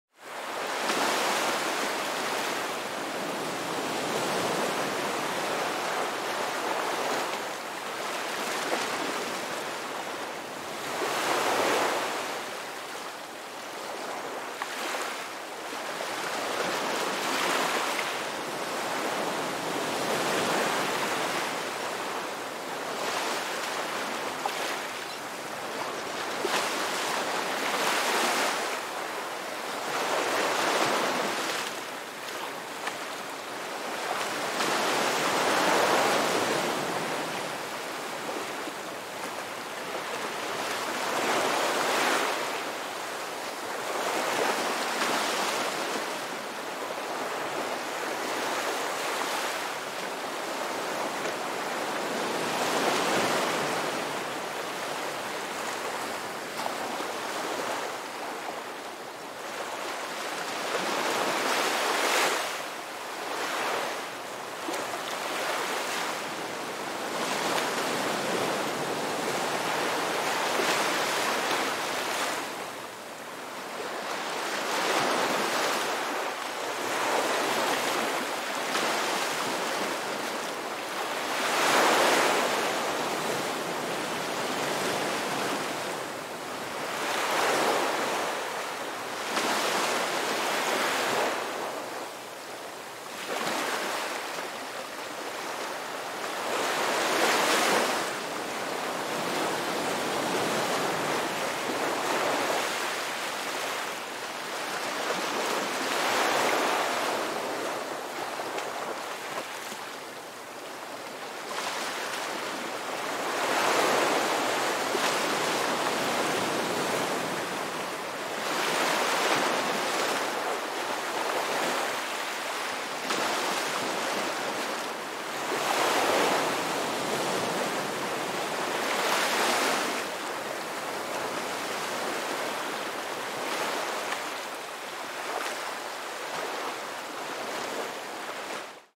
Oceans Alive
2 minutes, 18 seconds of crashing and rolling salty surface water, rolling and resting then churning and crashing again.